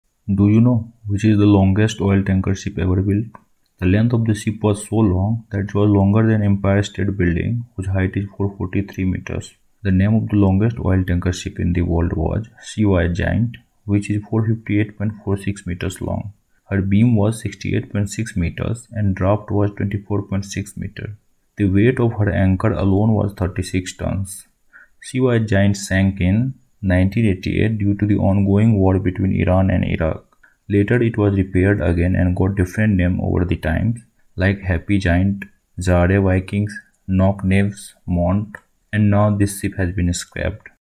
World's Largest Oil Tanker Ship Ever Built - How Big is It?
This video contains the information about the largest oil tanker ship in the world ever built.
The largest merchant navy oil carrier ship ever built.